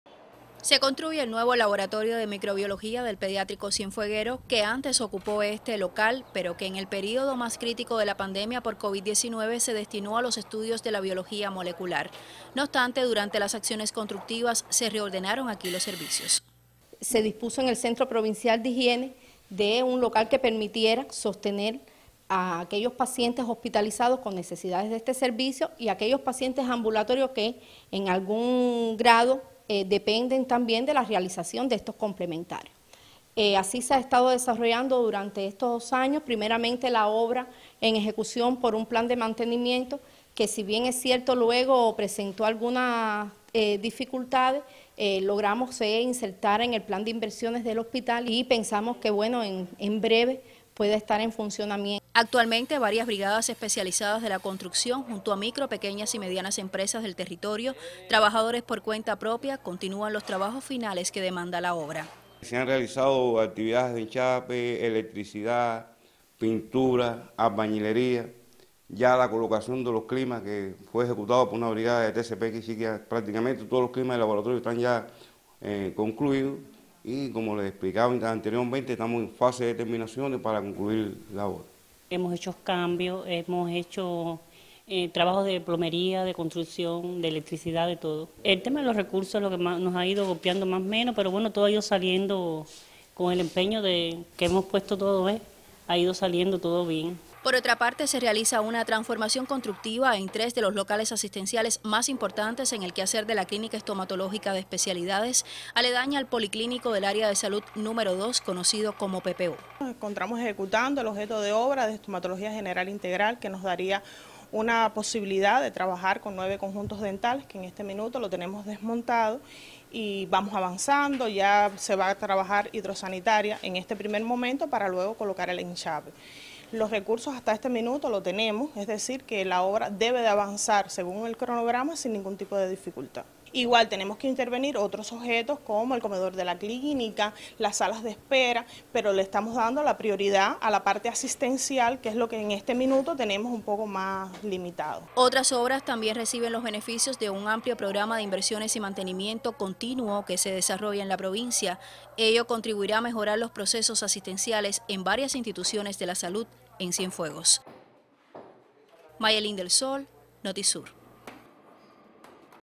Avanza en Cienfuegos construcción y mejoras de obras en sector de Salud
Acciones constructivas en varias obras sociales del municipio de Cienfuegos se ejecutan a buen ritmo, para ser devueltas a la prestación de los servicios a partir de este 26 de Julio. En los centros intervenidos se garantizará un mejor confort que garantice calidad en los procesos asistenciales.